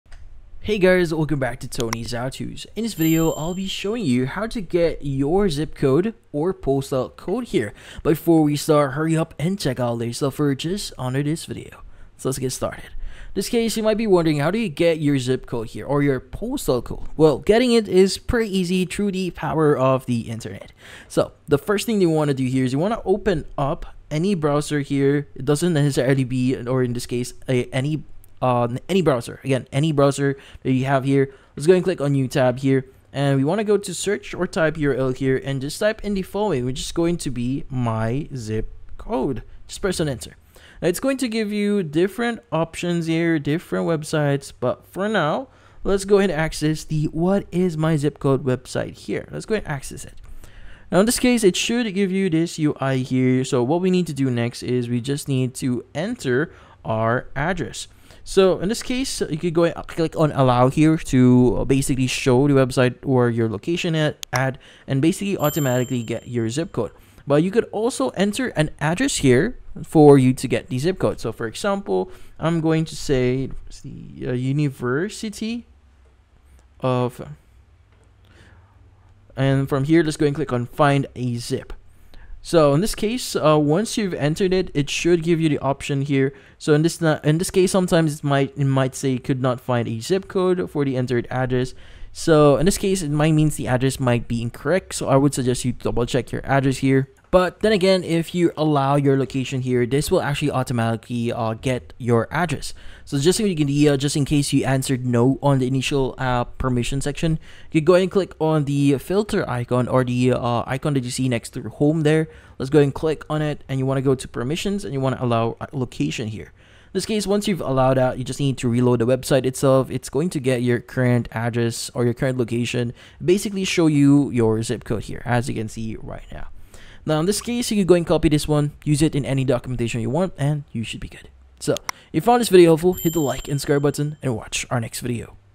How to Get Your Zip Code | Postal Code Search (2024)
In this video, I show you How to Get Your Zip Code | Postal Code Search in 2024. The quick and easy tutorial how to get postal code for apple id in sim registration. Do you want to know step by step how to know zip code for credit card in 2024? Yes? Great!